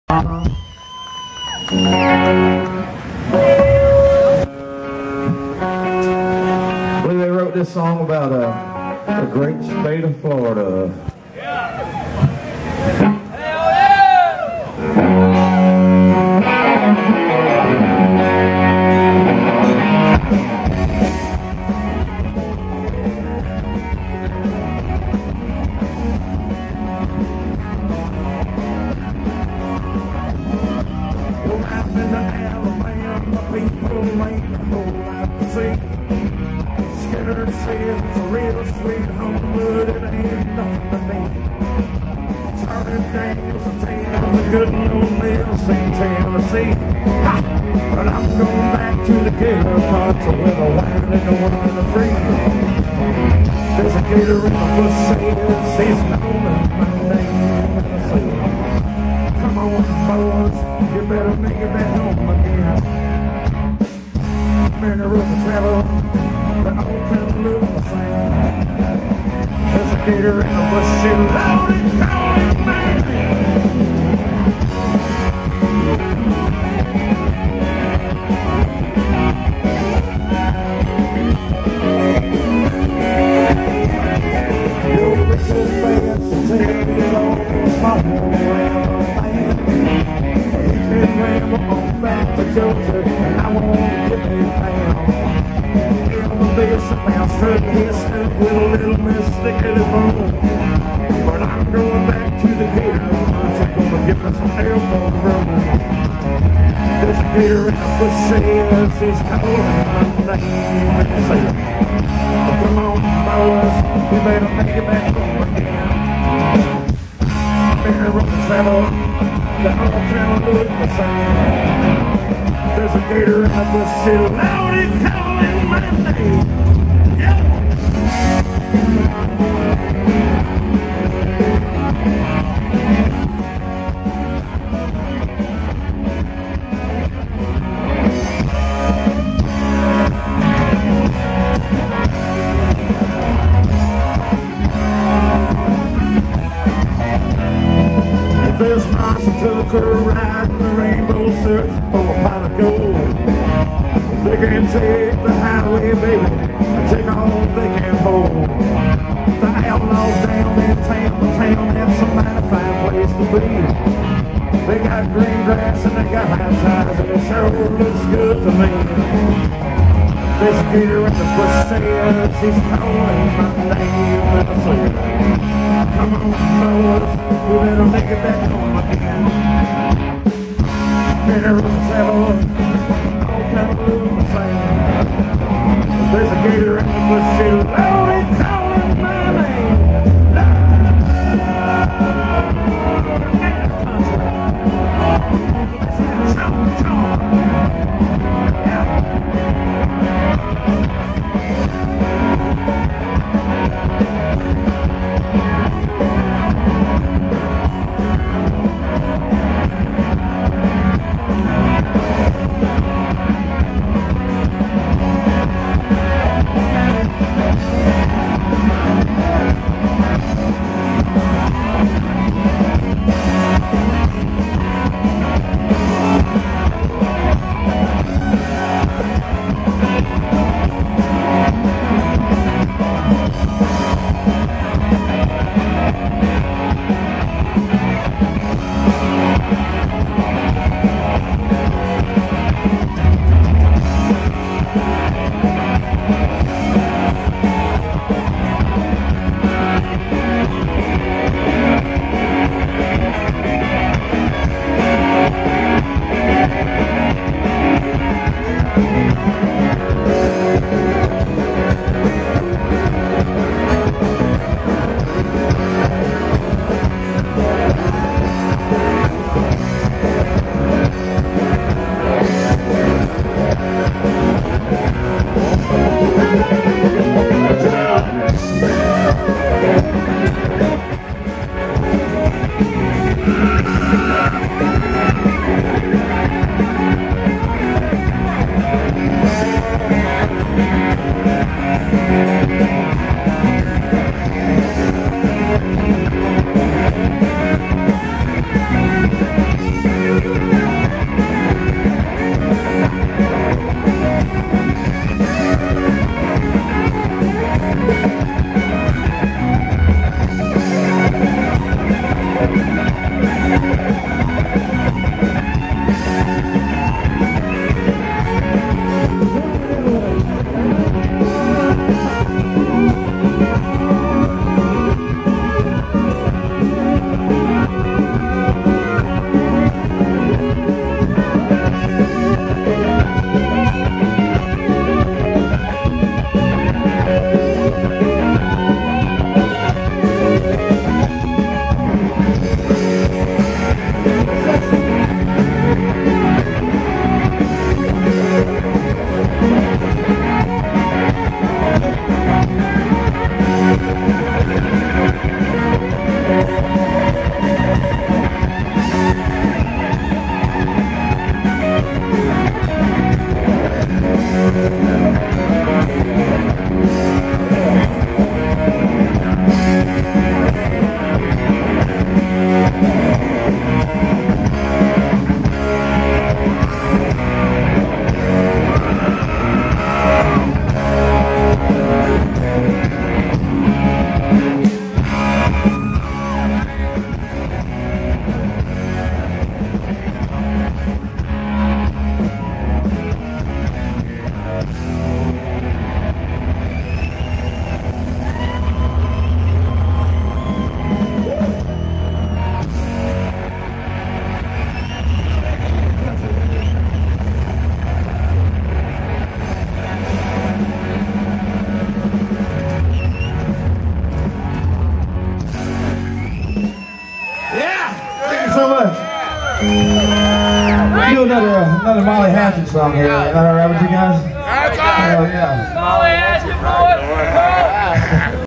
Gator Country By Southern Rock Legends at Seminole Casino Immokalee on 11-23-13
Banner Thomas on Bass, Charlie Hargrett on Guitar, Derek Hess on Drums, Jimmy Elkins on Vocals, Jimmy Sexton on Guitar, at Pigs N Paradise Festival, Video By Lana LeVande